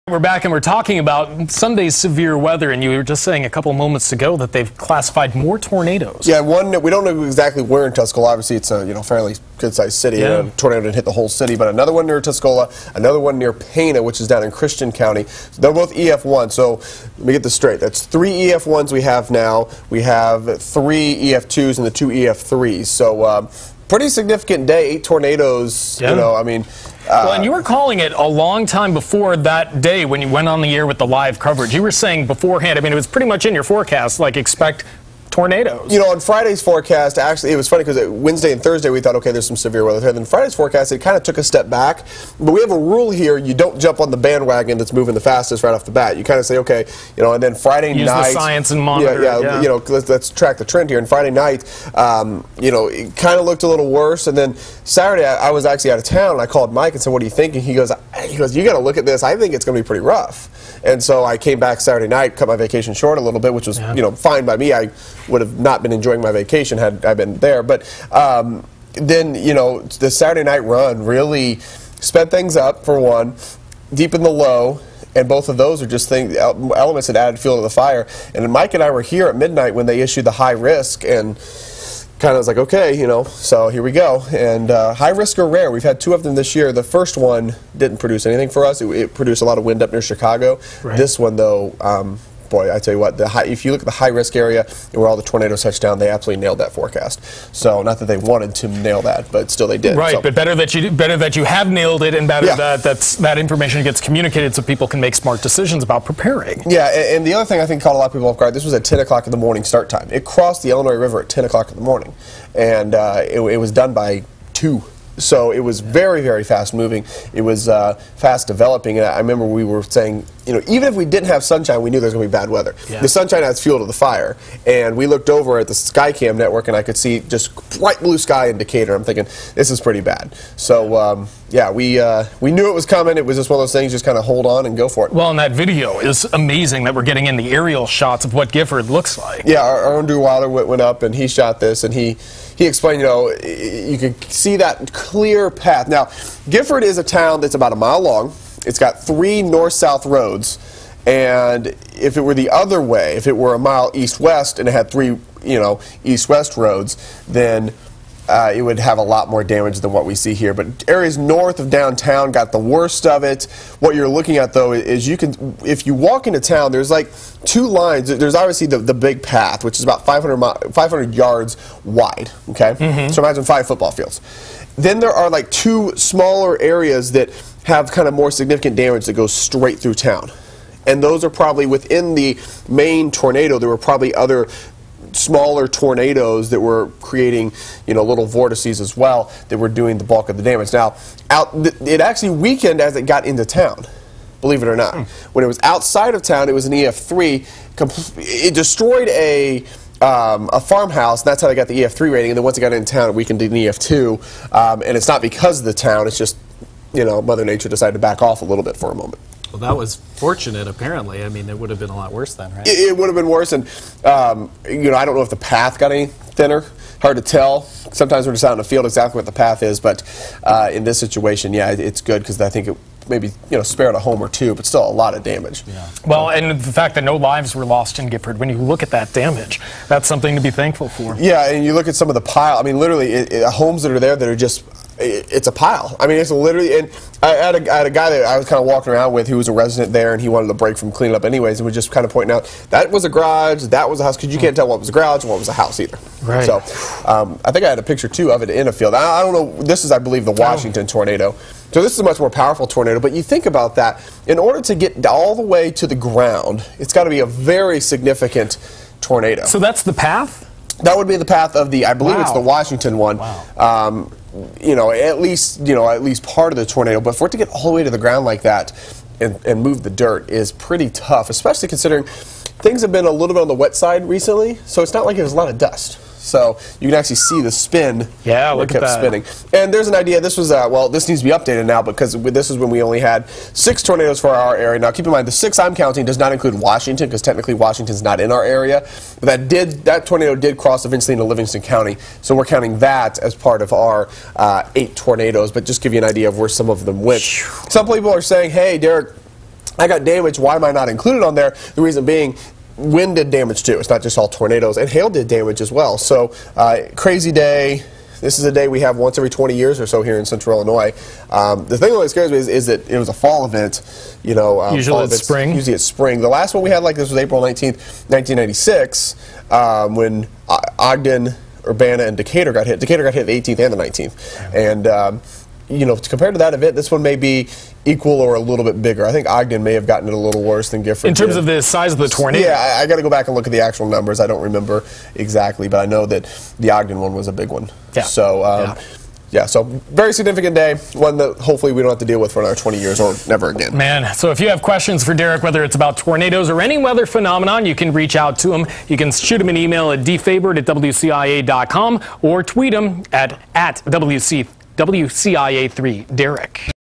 A Closer Look at the Gifford Tornado
WCIA 3 Chief Meteorologist Derick Fabert has an in depth look at the tornado that hit Gifford.